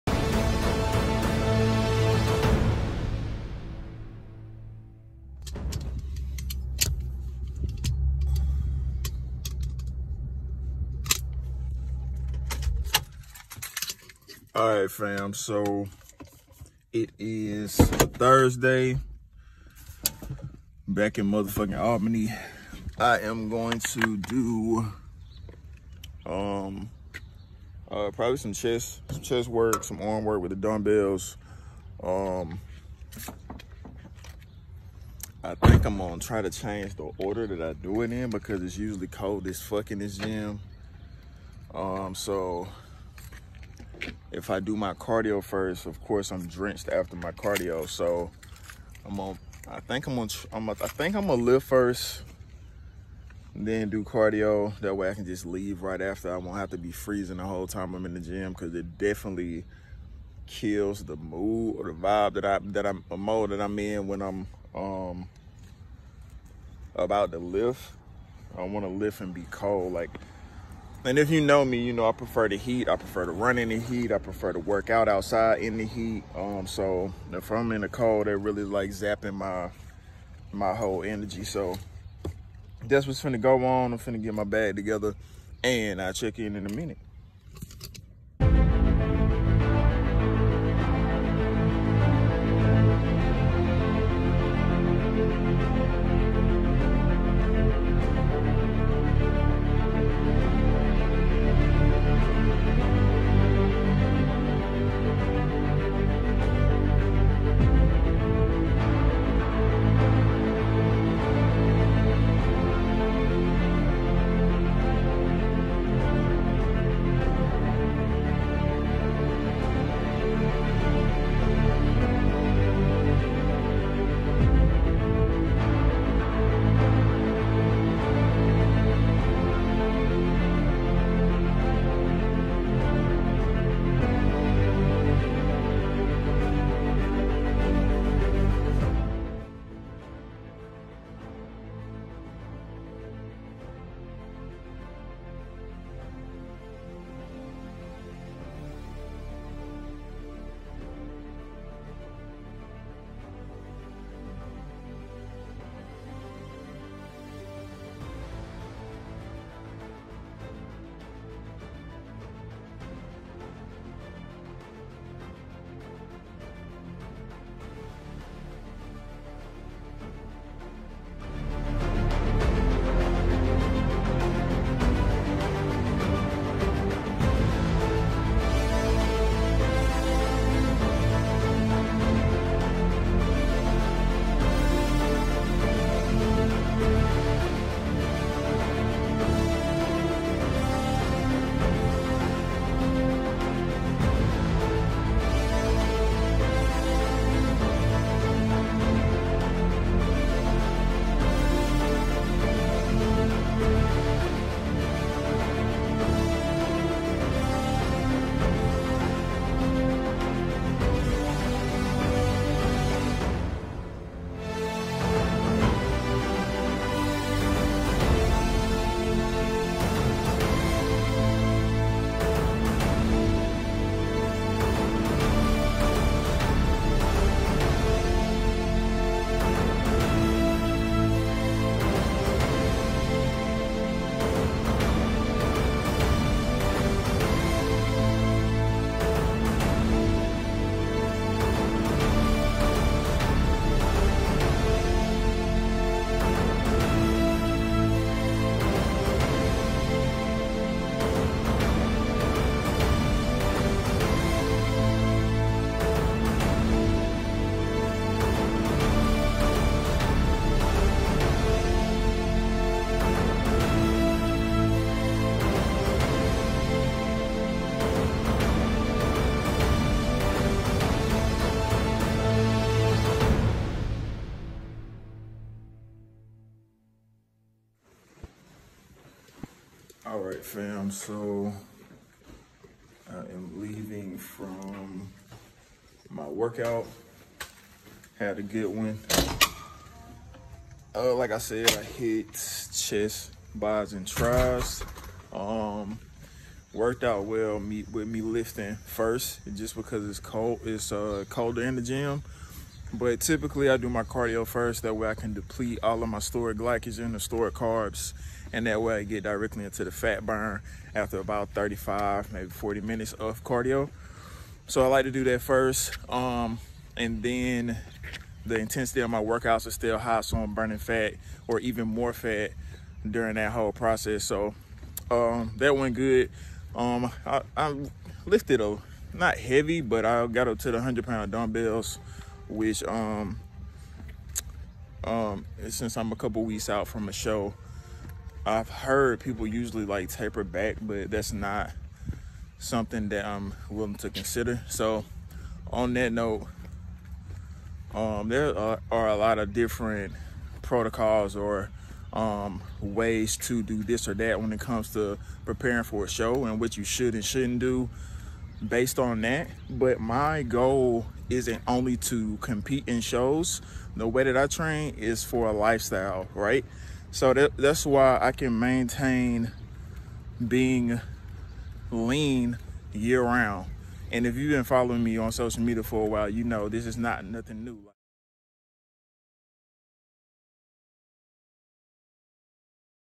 Chest Bi’s & Tri’s
In this video I give a look at a chest bis and tris workout I did where I’m hitting the 100lbs dumbbells while on prep. I also walk y’all through the layout of my workouts. Click any of the links below for a discount on Cellucor products.